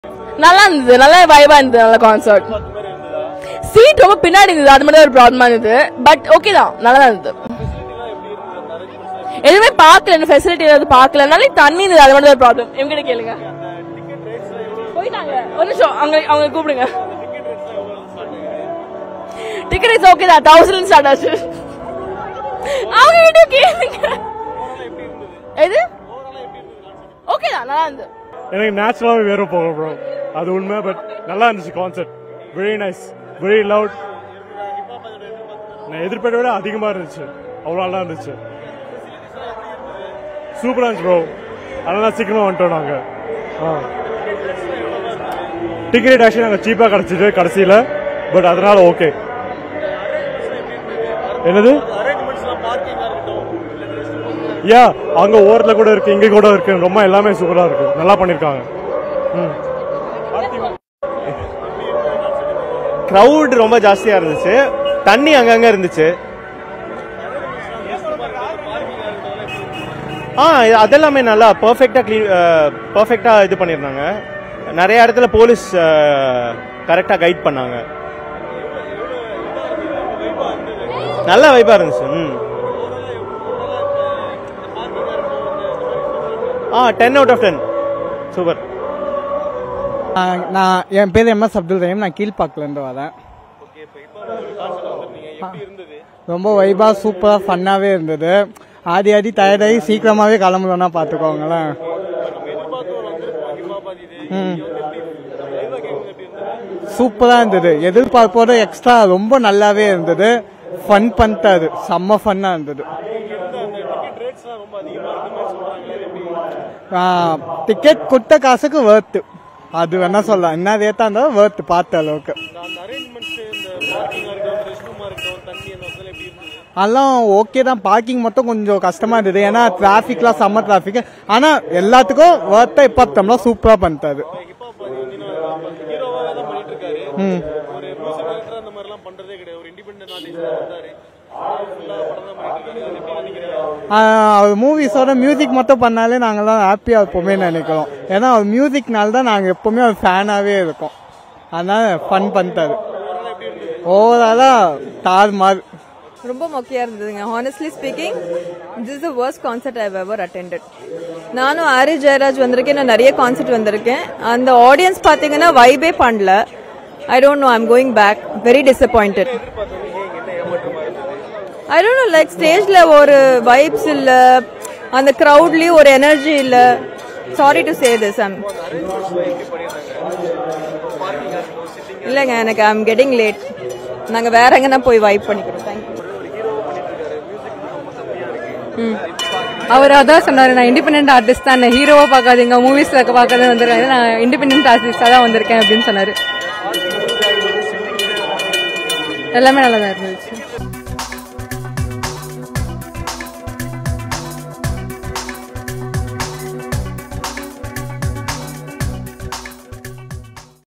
"செம்ம வைப் ப்ரோ.. சூப்பரா இருந்துச்சு".. ஹிப் ஹாப் ஆதி கான்செர்ட் வந்த ரசிகர்கள் கருத்து..!!
"செம்ம வைப் ப்ரோ.. சூப்பரா இருந்துச்சு".. படம் நடிக்கிறத விட மியூசிக் மட்டும் பண்ணா நல்லாருக்கும் - ஹிப் ஹாப் ஆதி கான்செர்ட் வந்த ரசிகர்கள் கருத்து